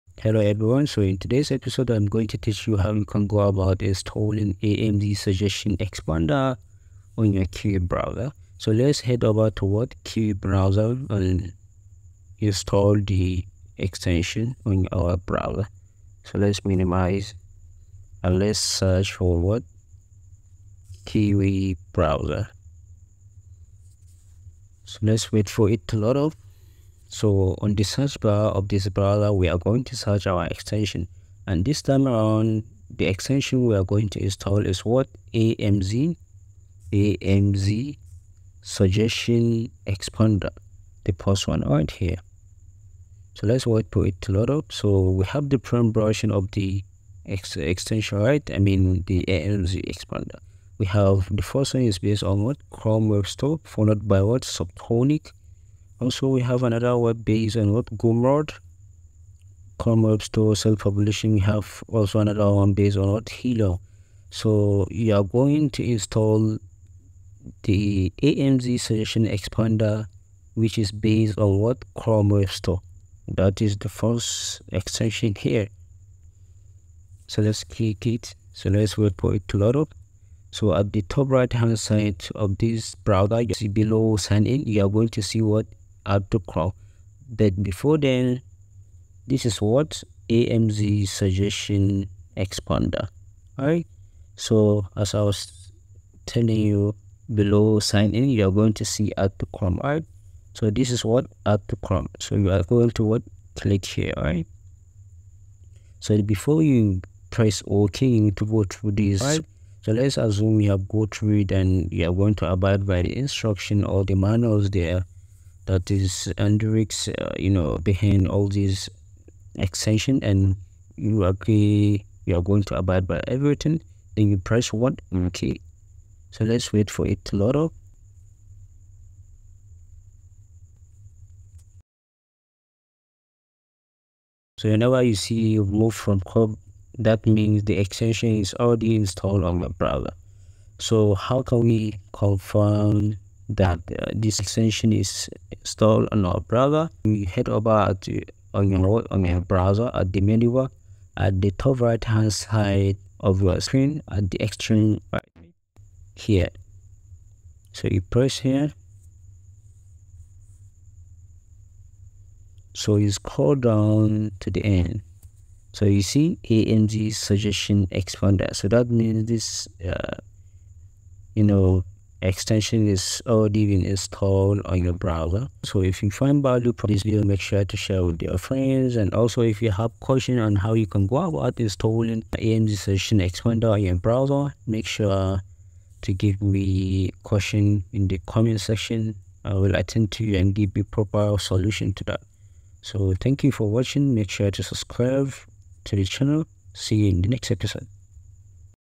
How To Install AMZ SUGGESTION EXPEANDER on your browser | kiwi & chrome
Amz suggestion expander is an extension that will help you do your research on obtaining keywordsbwith high demand and low supply. If you want to learn how you can install the tool profitably, watch this vidoe till the end.

This video is all you need to craft your professional and engaging book description for your Amazon KDP